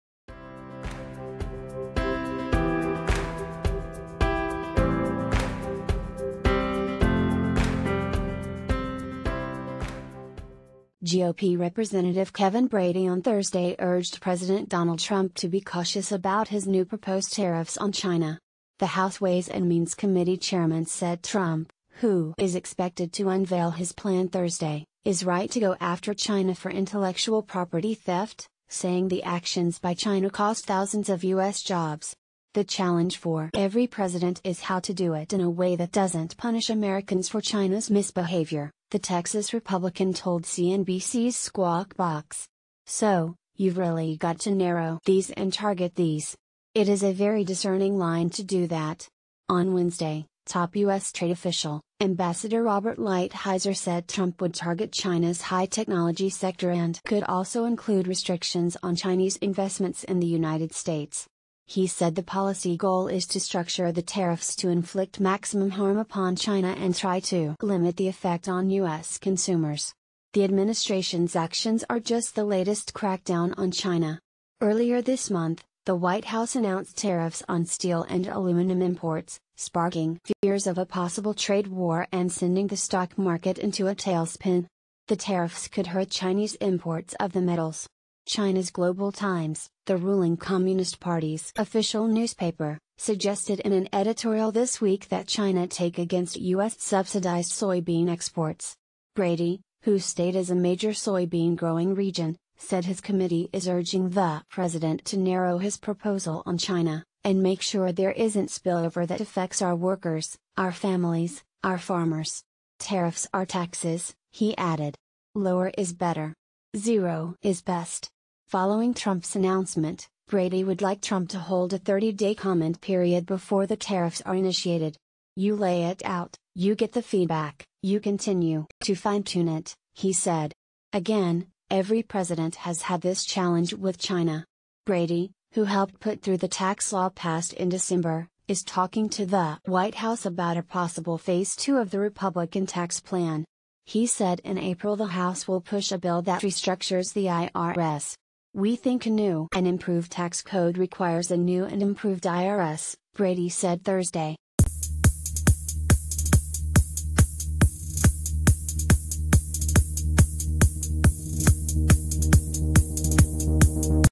[News] GOP's Kevin Brady to Trump: Don't blow tax cut gains with China trade war
GOP Rep. Kevin Brady on Thursday urged President Donald Trump to be cautious about his new proposed ...